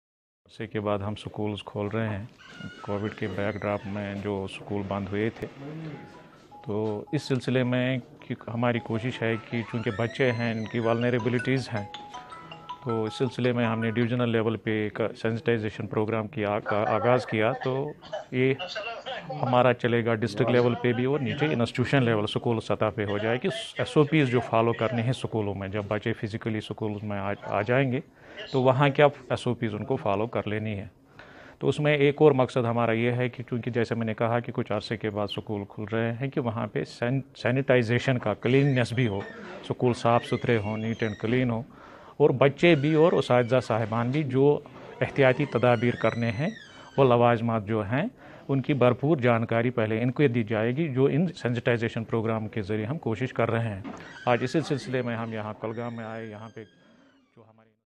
Director School Education Mohammad Younis Malik speaks on schools reopening
As the authorities have decided to open schools in valley and to make schooling safe and to keep COVID-19 at bay Director School Education Mohammad Younis Malik today visited Kulgam district and along with Deputy Commissioner kulgam Showkat Aijaz Bhat chaired a meeting of Officers here at Conference hall mini-secreterait kulgam.